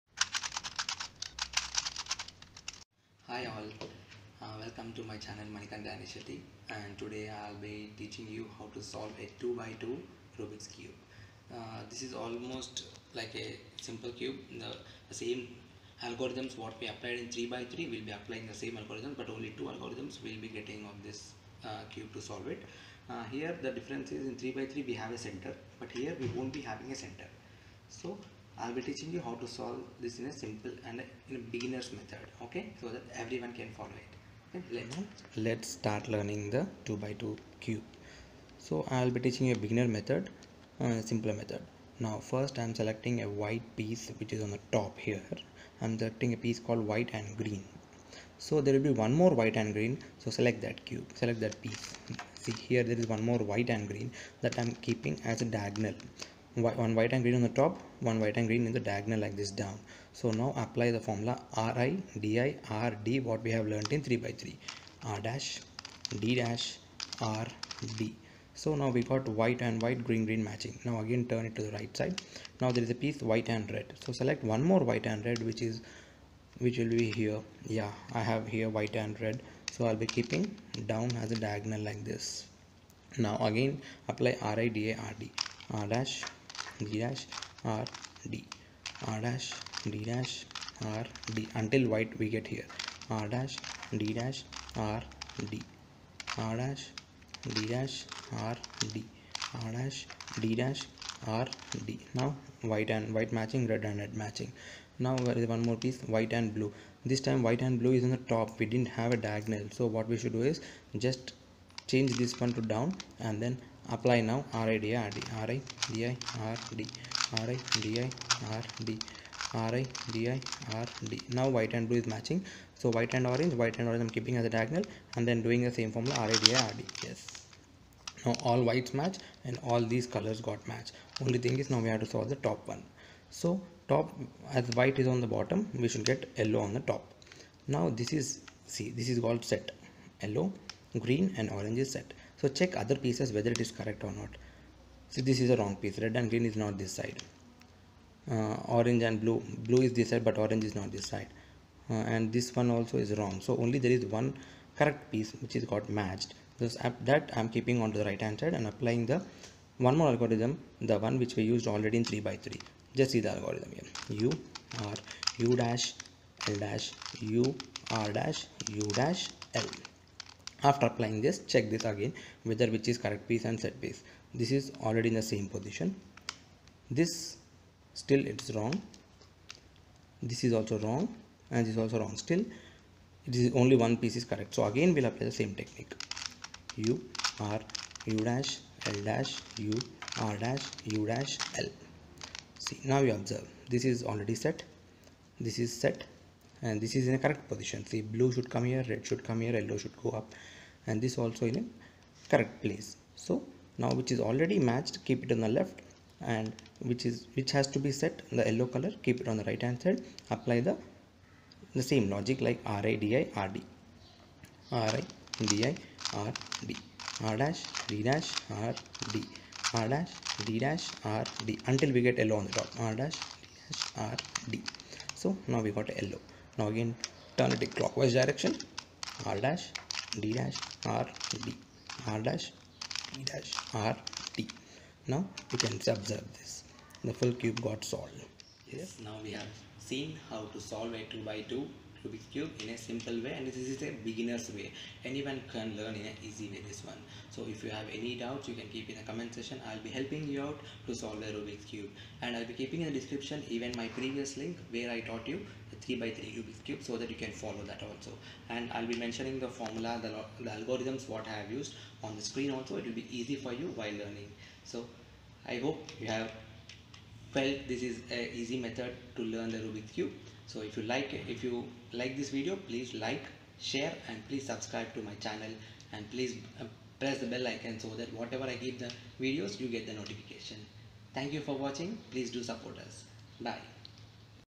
2 by 2 Rubik's Cube Tutorial | Beginner Method | Easy way to learn 2 by 2
To solve the cube follow these instructions
R - Turn right side clock wise.

Apply these two Algorithms
1. U R U' L' U R' U' L
2. R' D' R D